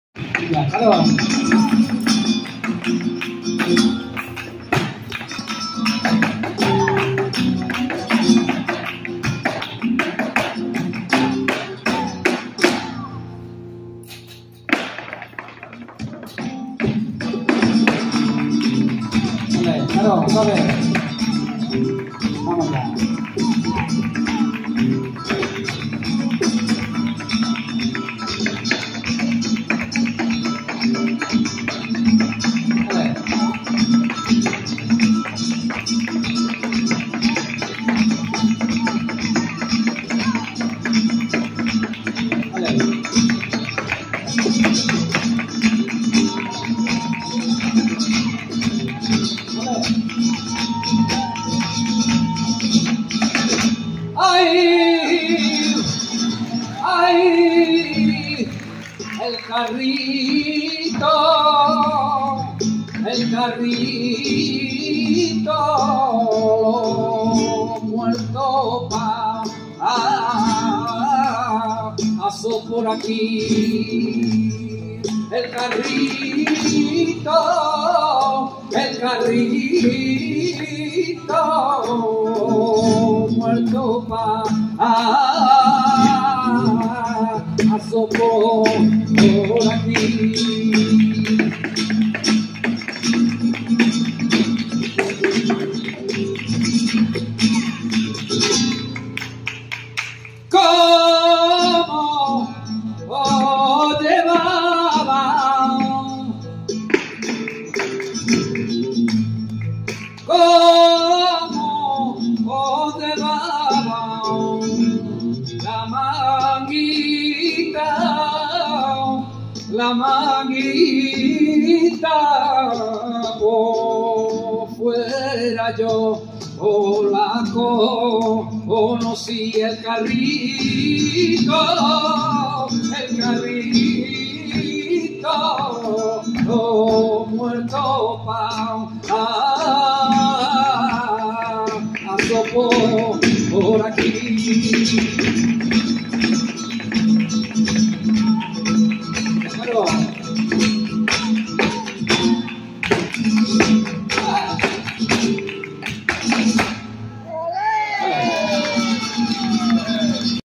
Carola Zertuche - Flamenco with a fan
with David McLean on guitar and Azriel Goldschmidt on vocals. Monthly Flamenco Friday performance at Rocky's Market on the Oakland estuary, 6/24/22